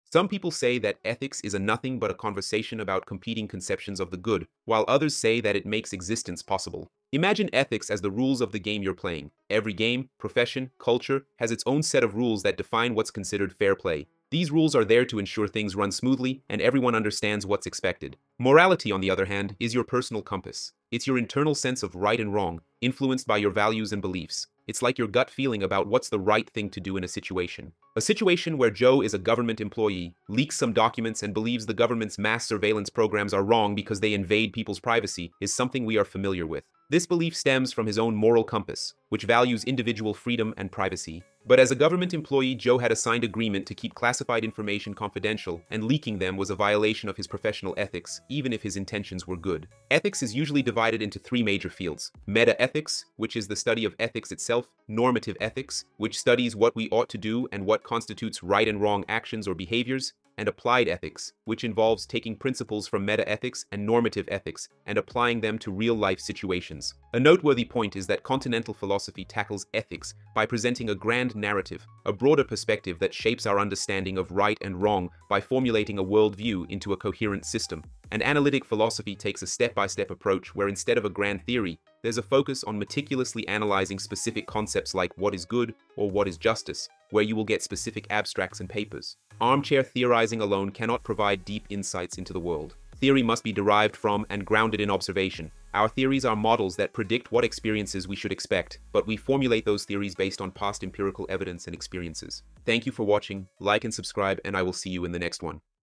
Ethics in 100 Seconds
Can you really understand ethics in just 100 seconds? I'll break down the core principles of ethics, exploring different philosophical perspectives and real-world applications. Whether you're a student, a professional, or just curious about this stuff, this quick video about ethics should leave you with some insights.